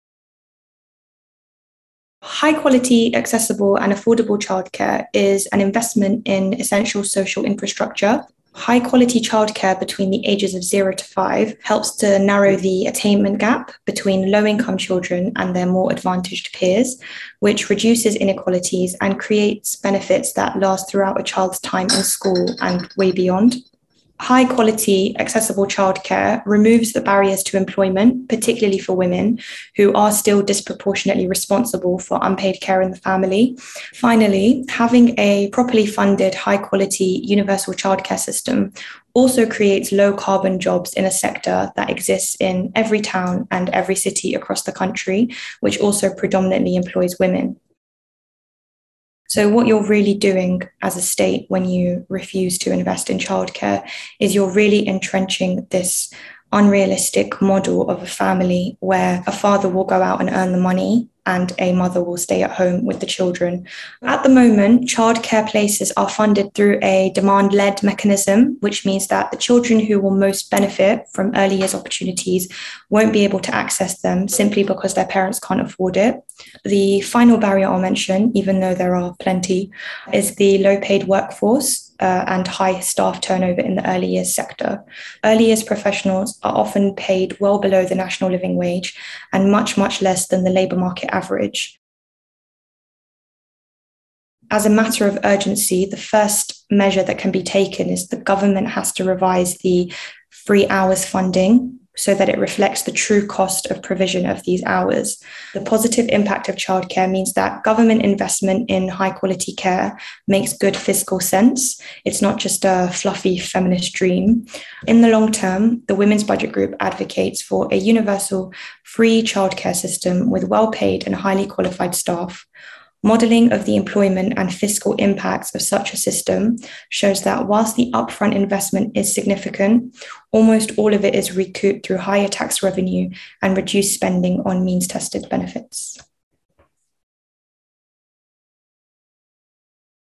A Social Guarantee for Childcare
Hanna Abid, Research and Policy Officer at the Women's Budget Group, tells us why childcare should be part of a Social Guarantee.